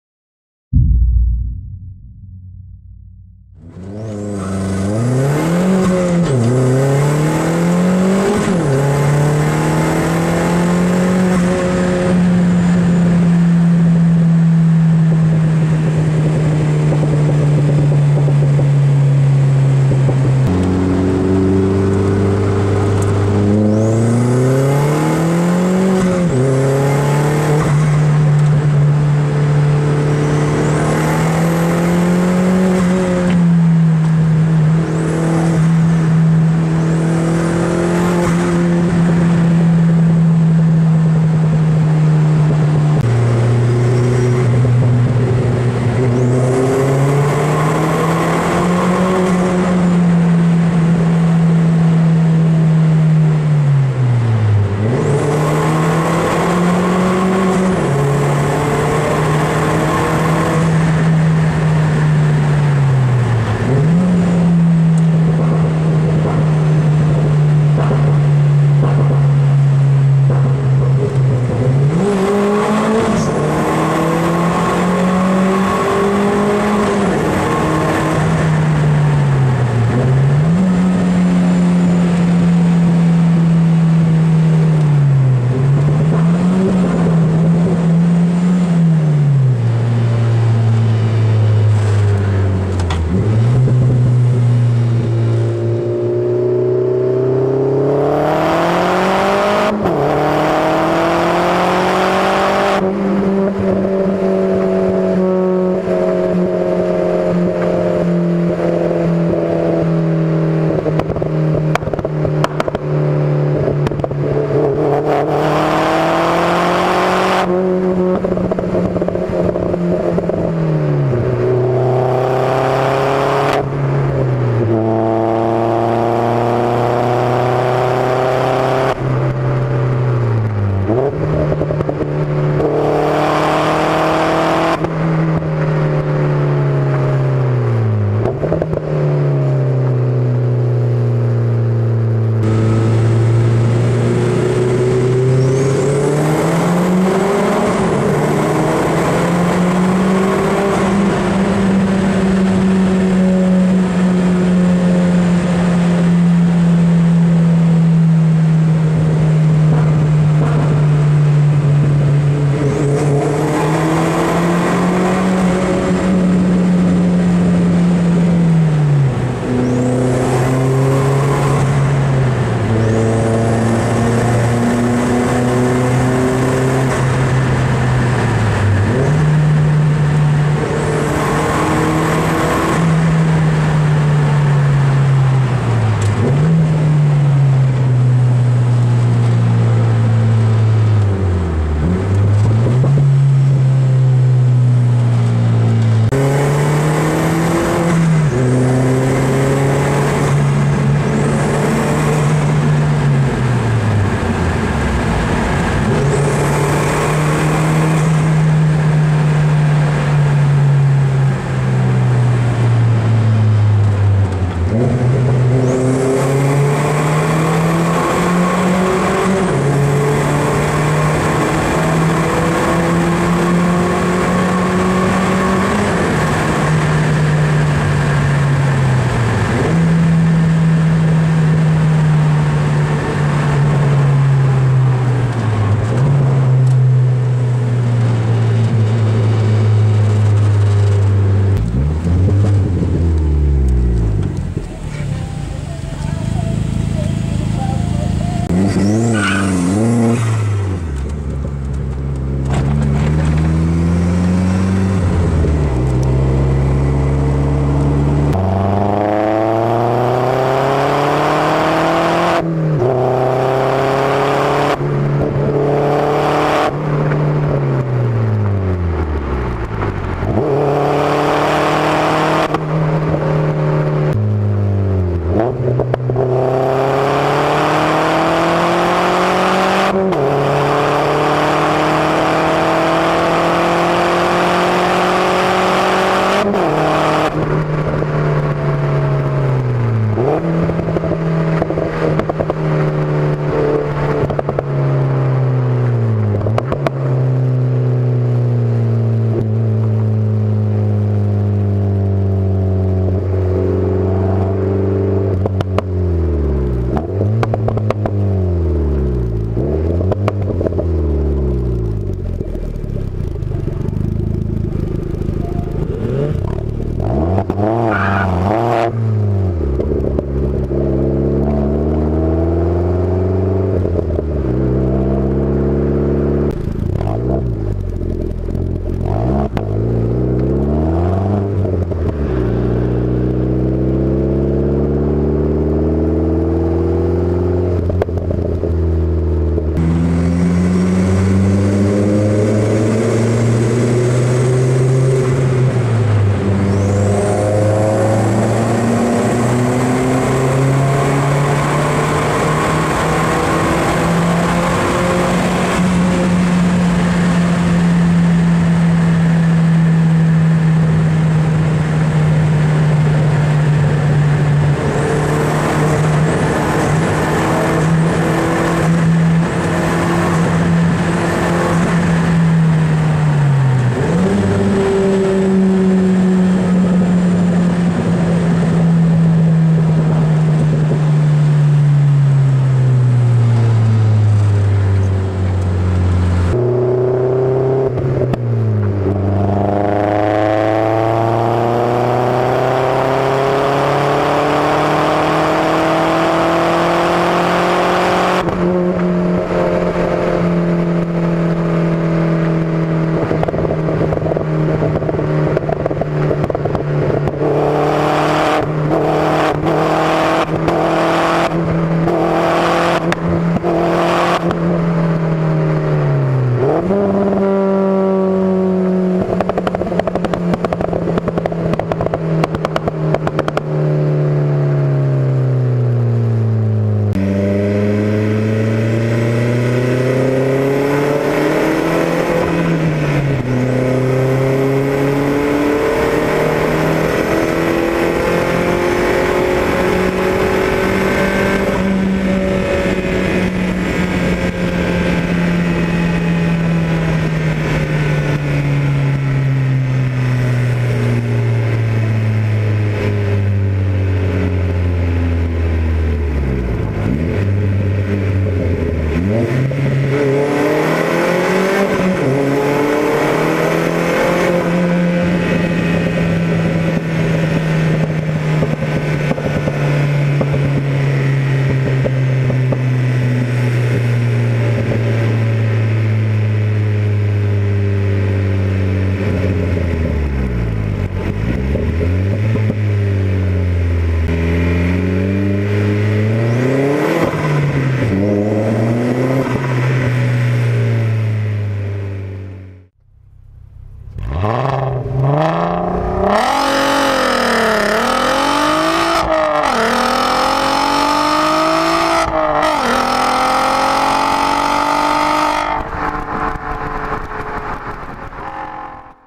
Modified Opel Speedster Turbo: Tunnel sounds, Onboard, Gopro Exhaust
INFO BELOW!
This time I went for a drive with a friend and his highly modified Opel Speedster Turbo. Enjoy the video!